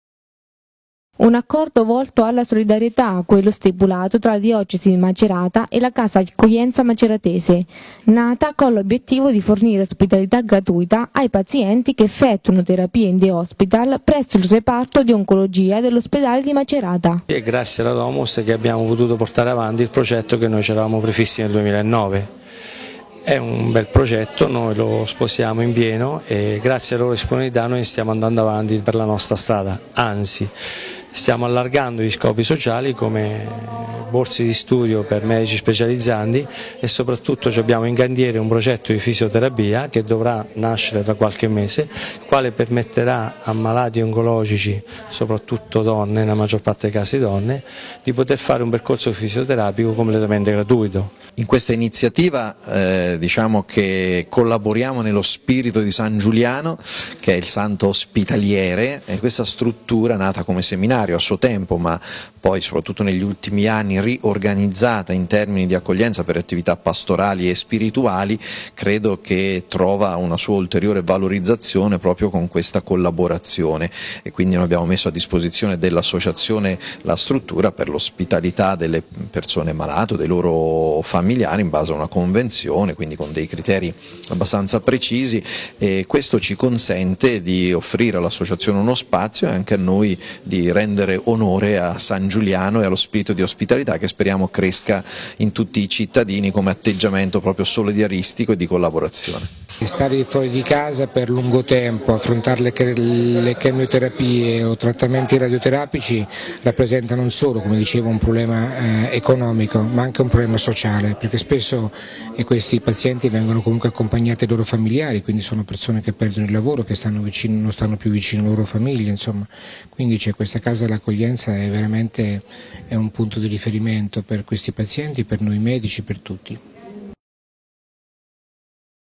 MACERATA. FIRMATO ACCORDO PER L'ACCOGLIENZA DEI FAMILIARI DEI MALATI
Macerata. 7 novembre 2012. È stato firmato stamattina l' accordo tra Diocesi di Macerata e Casa Accoglienza Maceratese per offrire ospitalità gratuita ai pazienti di Oncologia dell'Ospedale di Macerata e ai loro familiari nel'intento di garantire solidarietà a quanti si trovano ad affrontare difficoltà legate alla malattia. Servizio di Elisabetta Tiracorda.